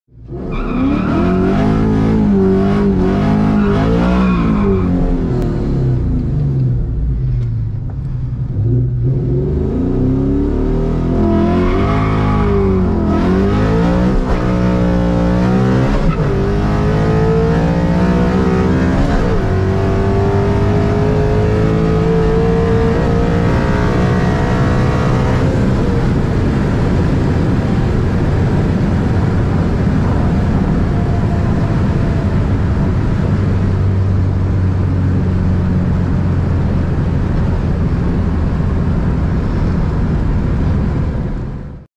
Coyote 03 Mach 1,Dragy hit..
Trying out the GoPro head mount..Another 12.2@120 on the dragy 🤙🏻